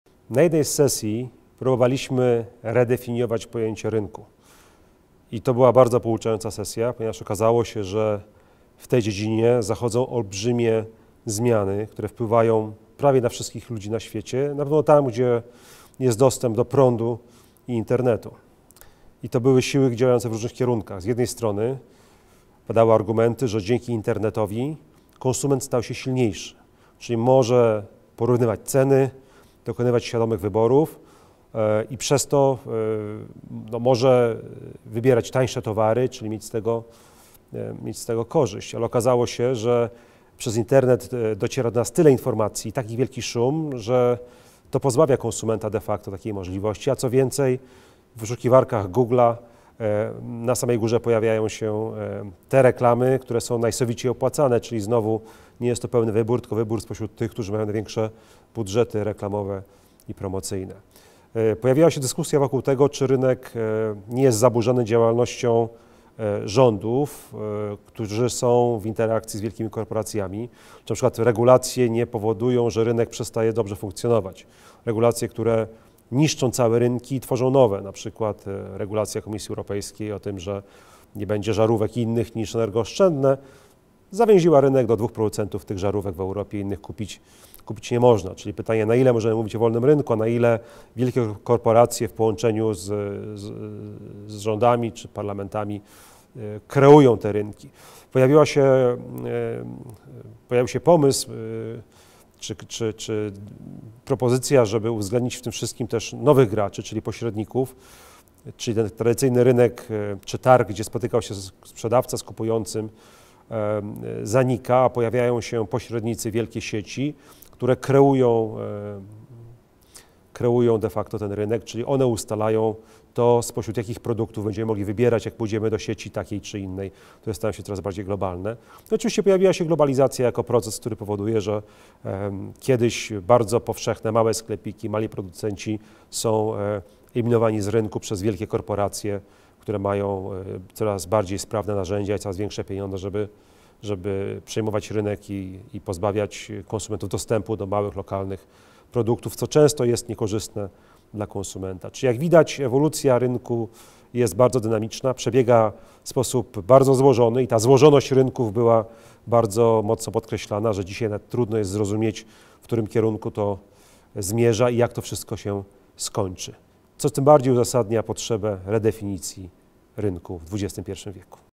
Krzysztof Rybiński
Czy żyjemy dziś w dyktacie rynku i na ile możemy mówić o realnym wolnym rynku? O tym mówi Krzysztof Rybiński, ekonomista, rektor Uczelni Vistula.